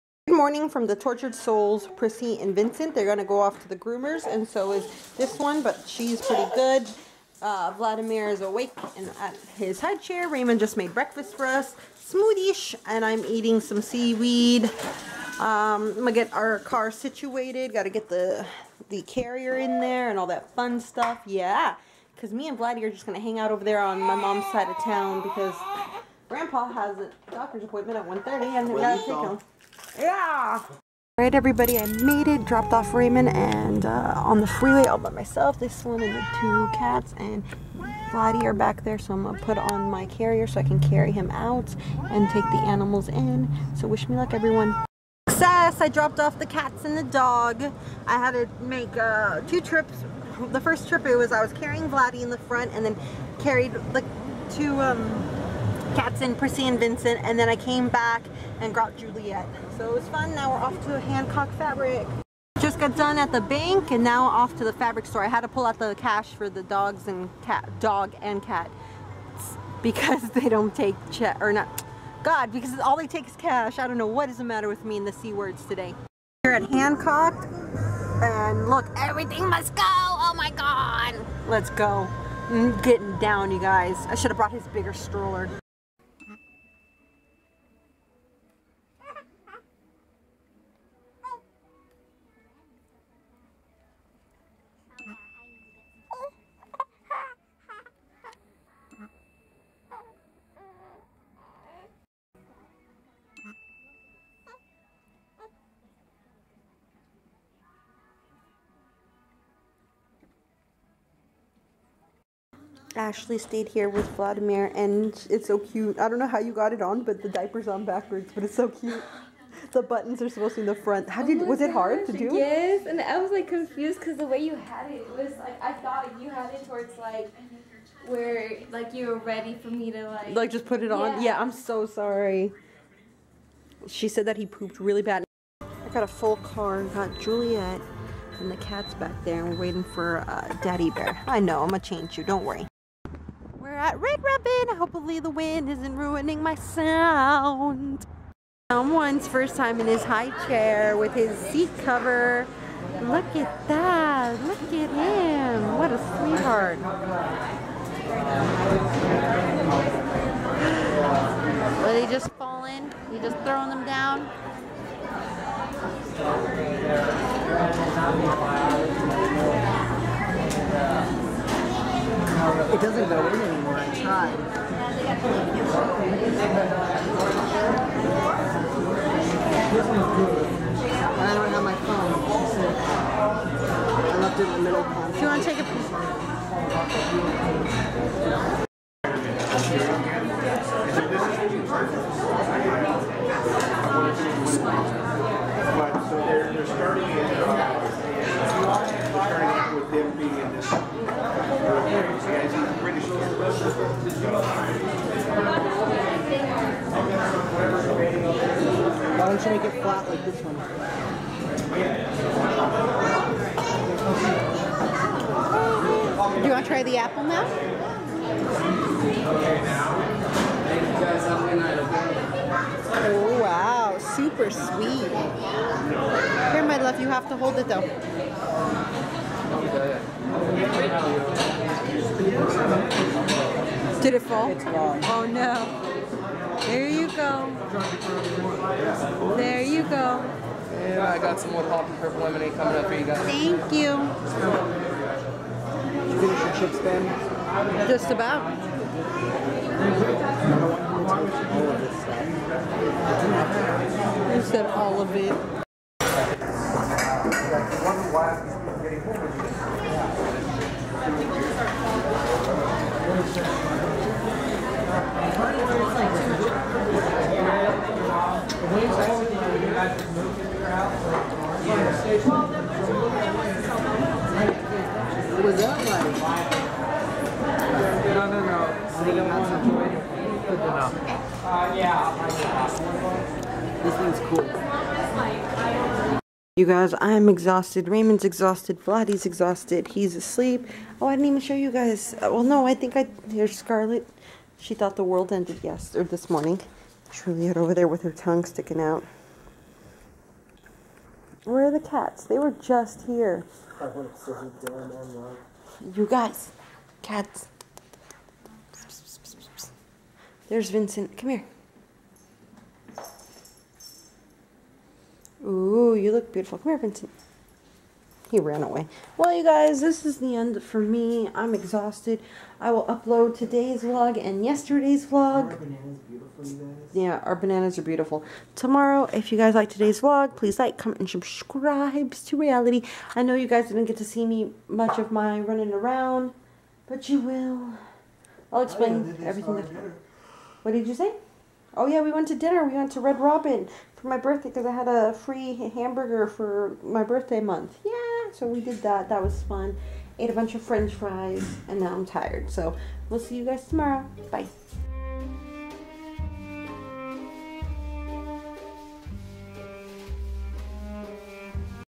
Using a cup?! [04.07.16] Day 357
Finally picked up fabric for my Princess Peach costume and Vlad tries out a cup!